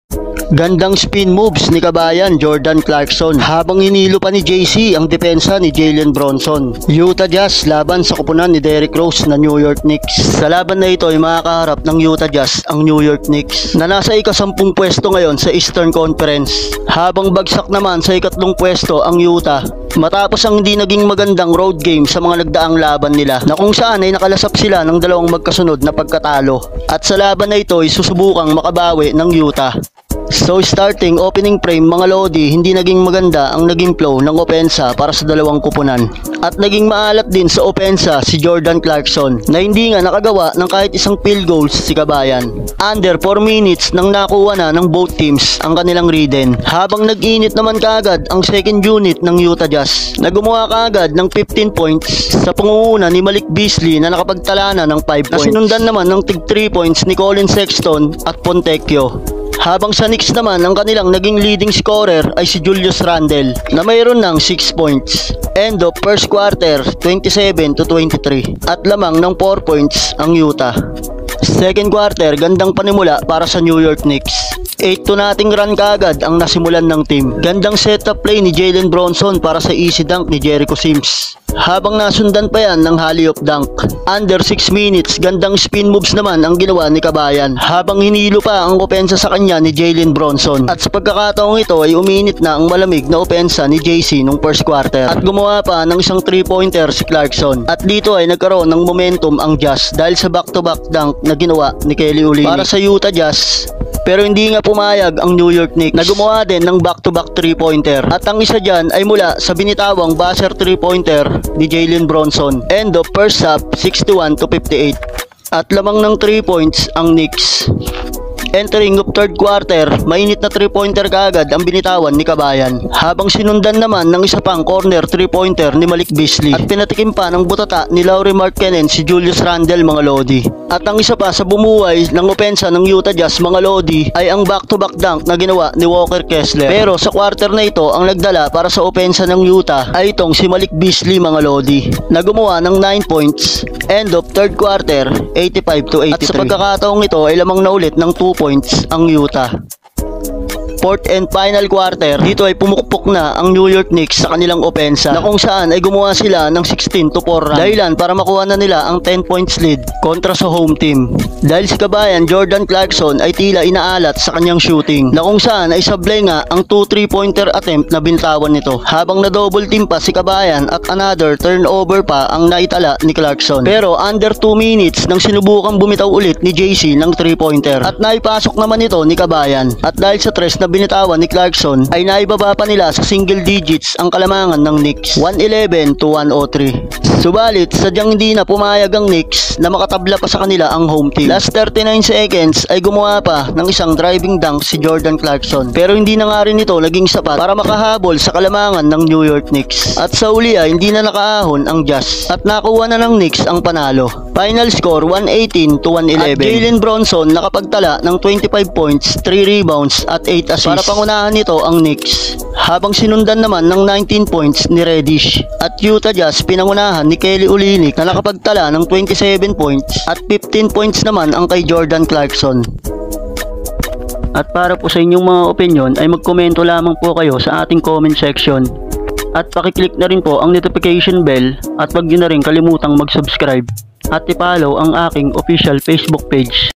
Grabe! Gandang Spin Moves Ni Jordan Clarkson Nahilo ang bantay na Si Jalen Brunson | Utah Vs Knicks
Grabe Gandang Moves Ni Jordan Clarkson Nahilo Ang Bantay Sa Ginawa Ni JC Utah Jazz Vs NeWyork Knicks